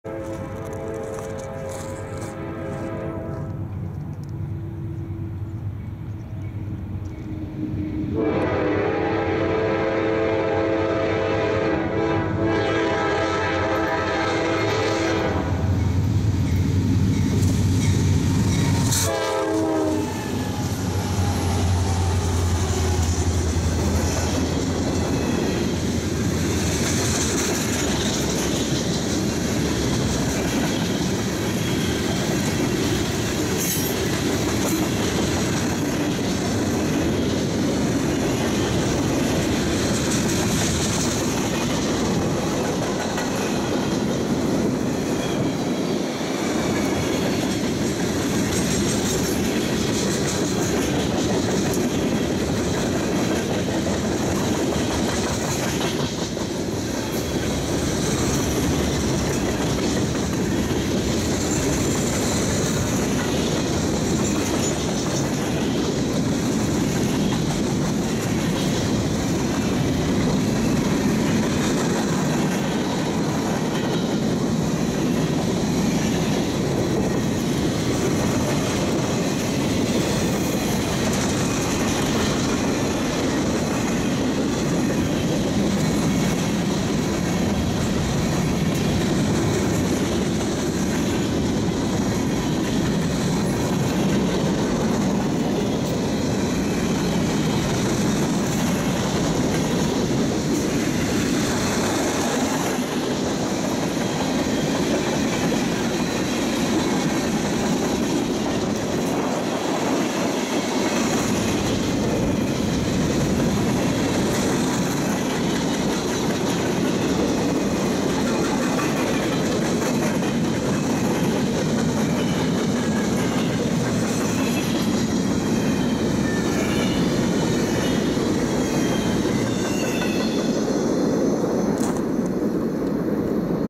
NS: A mixed freight train @ the AMTRAK station, Spartanburg SC
A mixed freight southbound is passing by the AMTRAK station in the late afternoon sun.
Video©trekky60